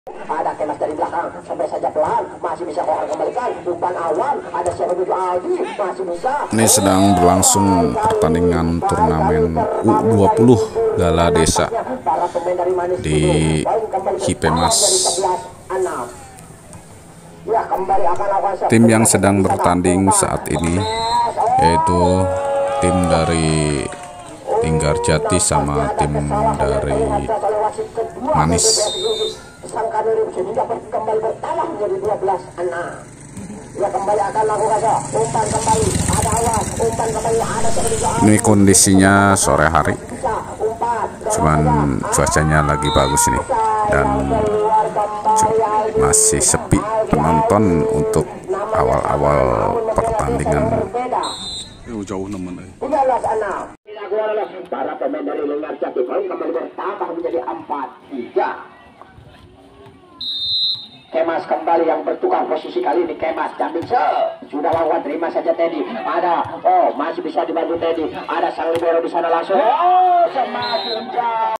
pertandinga bola voli gala des u20
sedang menonton pertandingan bola voli gala desa u20